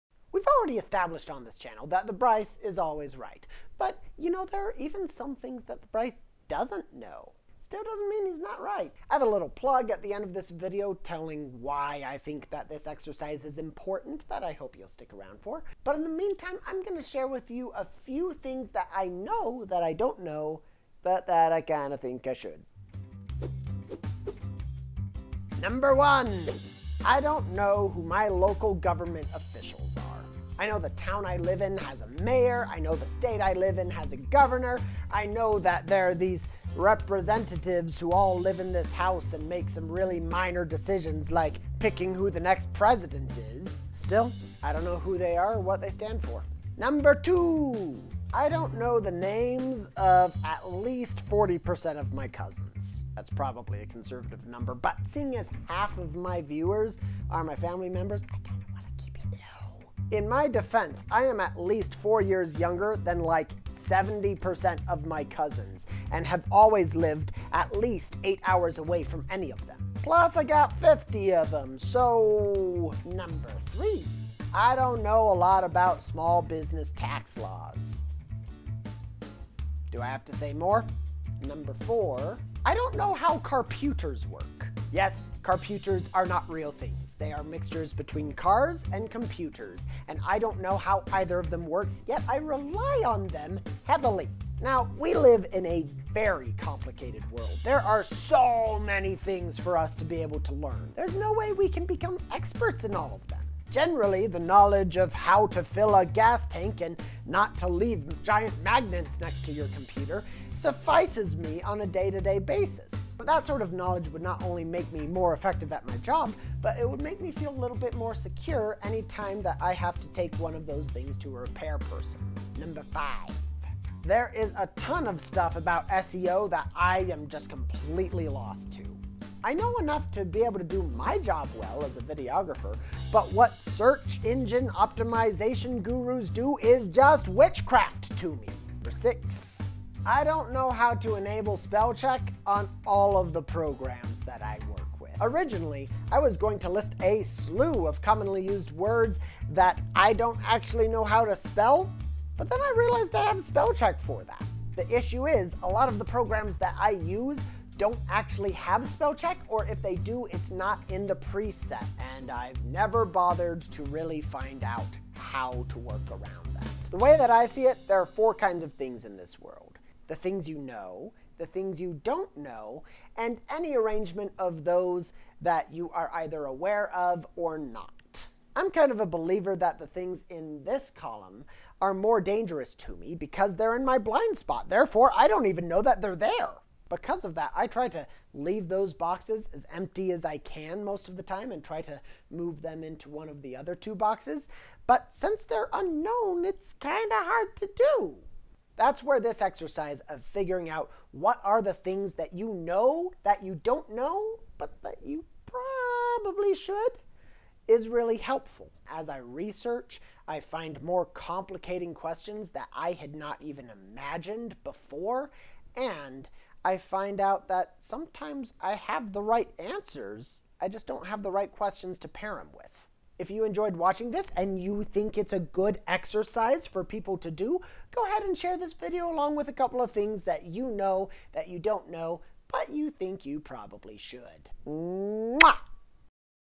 6 Things I Should know but Don't
What are some things that you know that you DON'T know, but that you probably should?